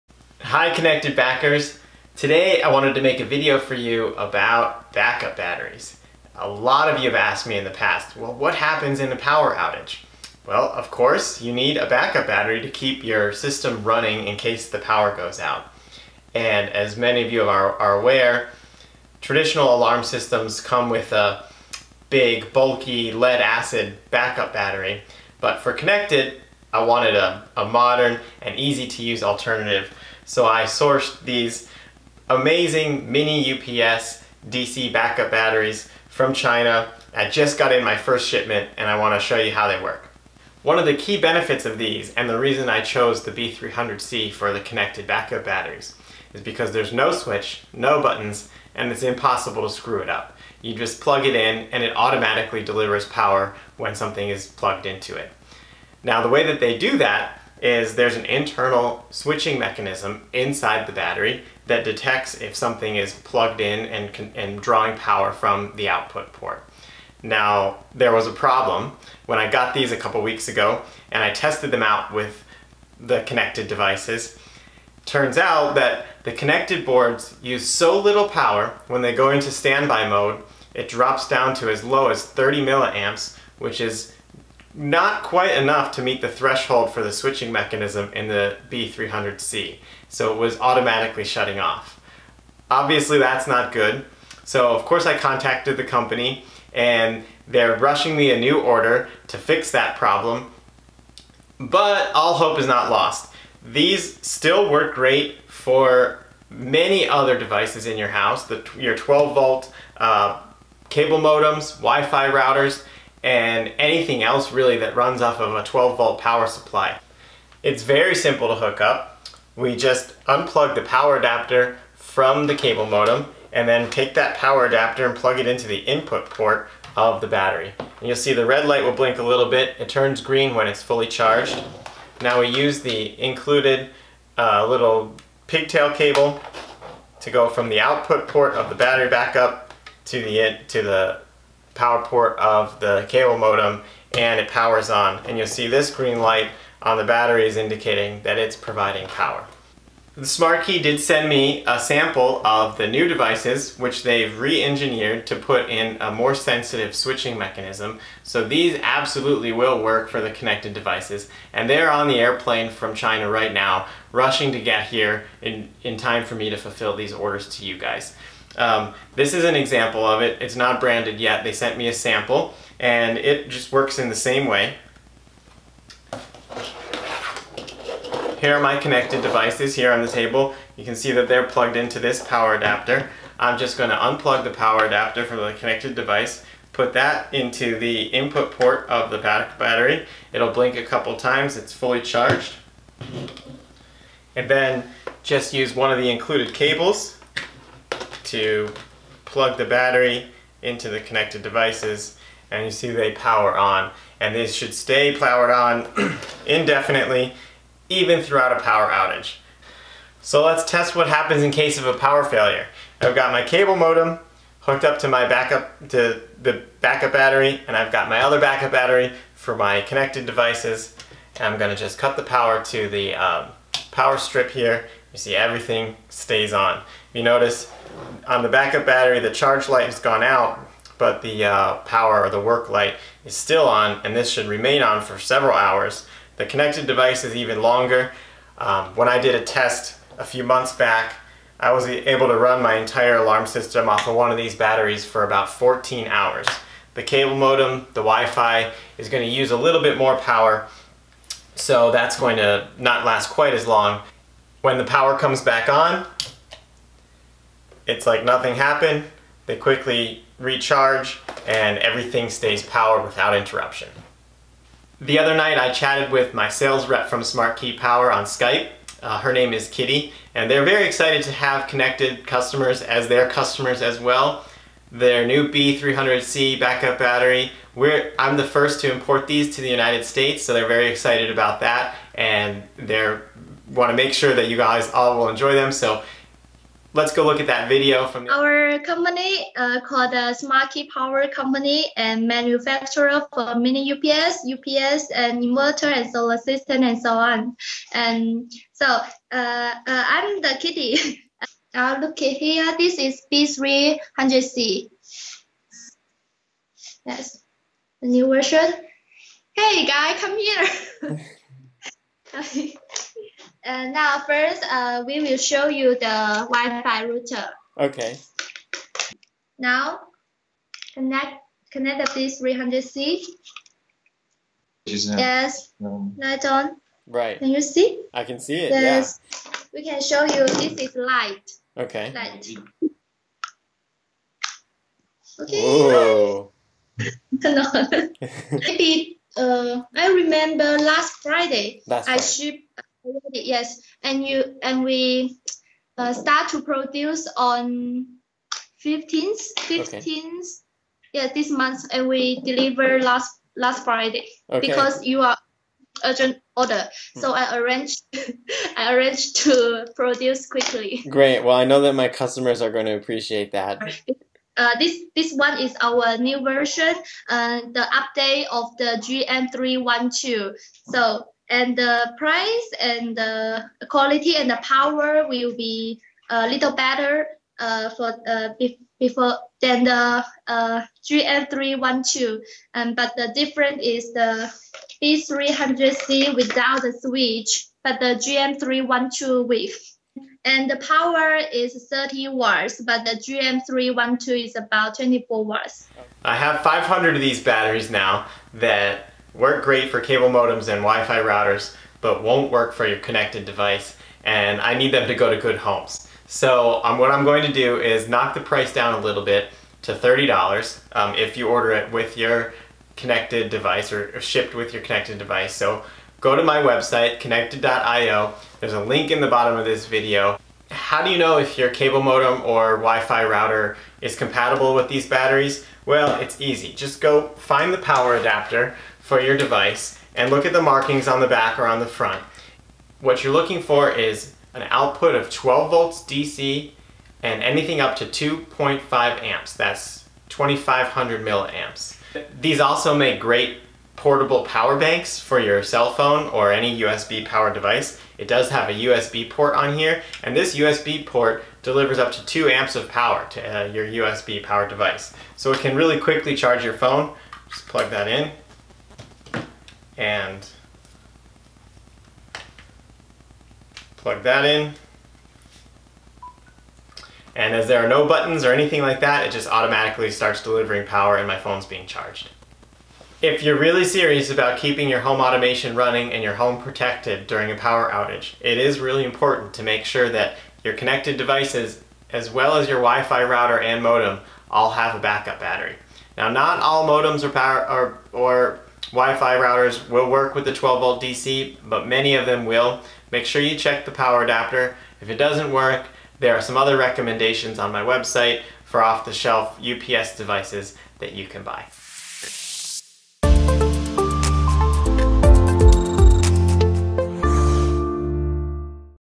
Konnected Backup batteries
In this video I demonstrate the new Konnected backup batteries (model B300C) that just arrived from Shenzhen SmartKey Power Co.

Due to a little specifications error explained in the video, for a limited time we have TWO variations of this battery:

Konnected Backup Battery (SKU: BAT-B300C) for Konnected devices -- $33

Konnected Backup Battery (SKU: BAT-B300C-X) for WiFi router, modem, or Raspberry Pi only -- SALE $30

The older version (GM312) from the same manufacturer is also available at:

Amazon US
Amazon CA
Amazon UK